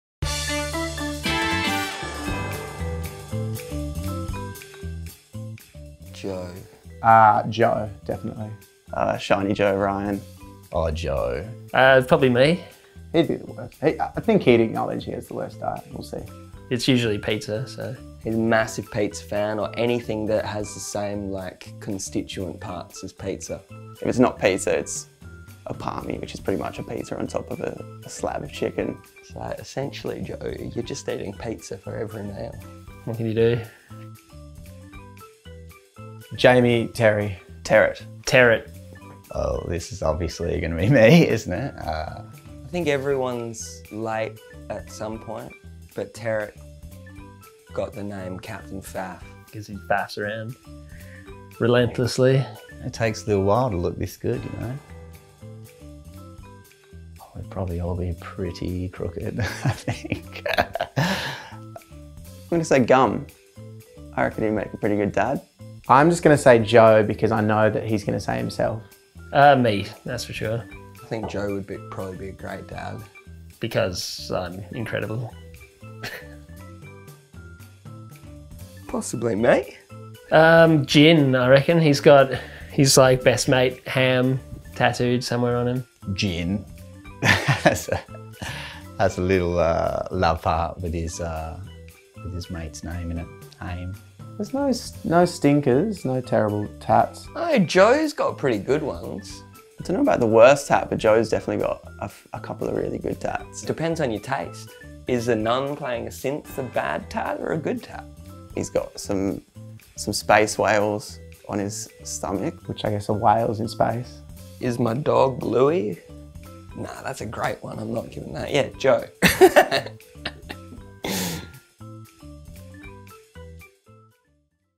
How Well Do The Members Of POND Know Eachother? | LNWY
Ever wondered how well the members of POND know each-other? We put them to the test.

read. watch. listen.